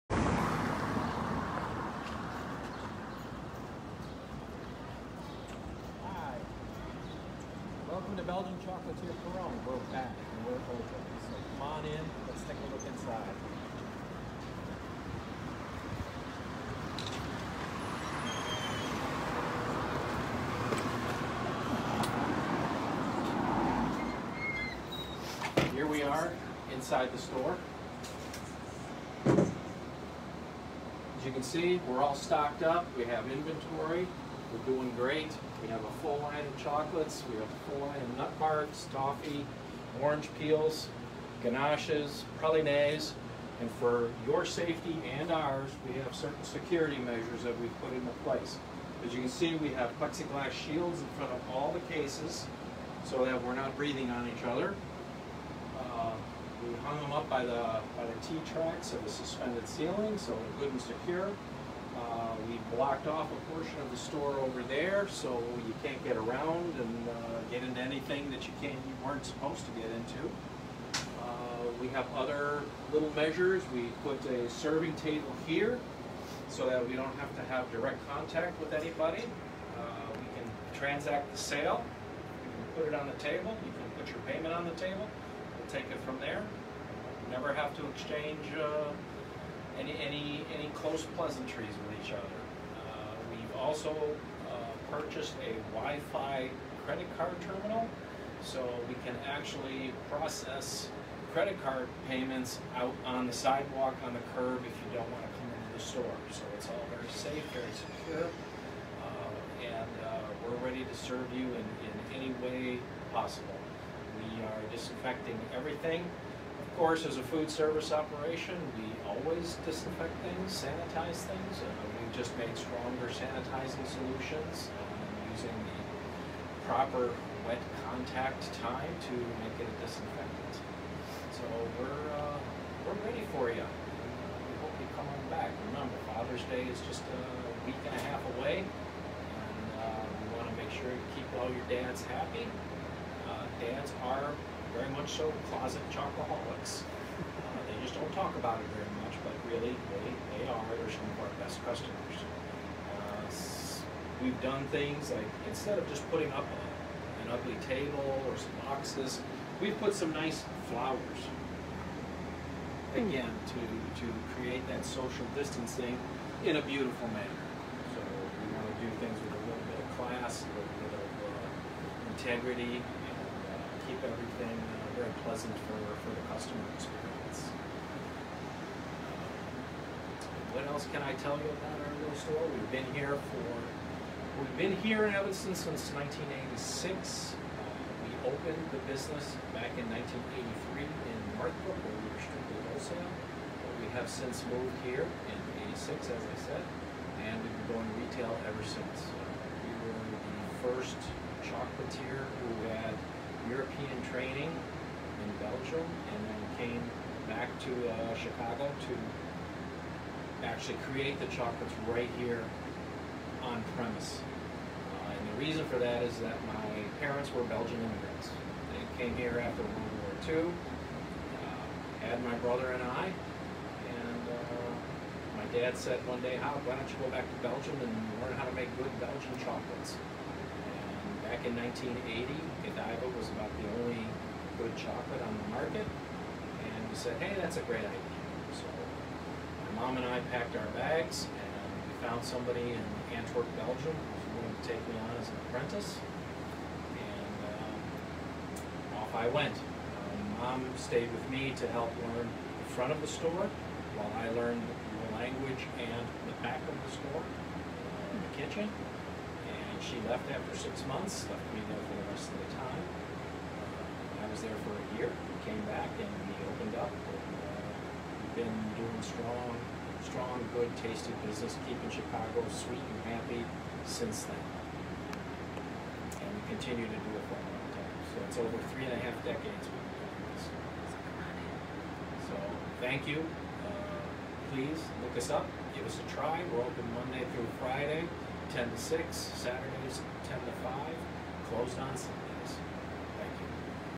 Grand Re Opening with Belgian Chocolatier Piron
Grand Reopening at Belgian Chocolatier Piron, Inc.! Bob Piron, Patti Piron and Fred as the owners are happy to run an essential business - CHOCOLATE! Curbside, instore, pickup, local delivery, and the doors are open. Masks required... no reason not to get your chocolate fix!

4 June 2020